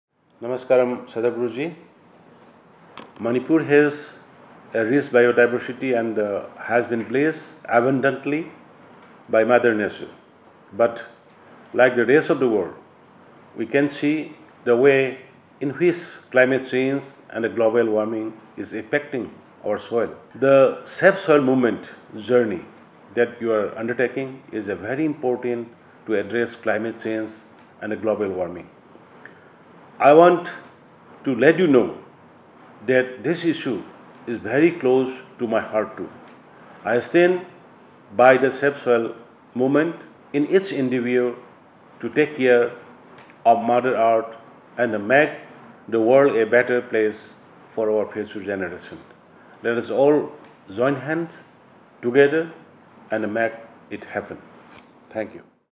Let Us Join Hands To Support The 'Save Soil' Movement, An Initiative By Sadhguru Ji
Similarly, the Government of Manipur has been taking up various measures to revive our soil and move towards organic farming altogether.

Published originally on May 13, 2022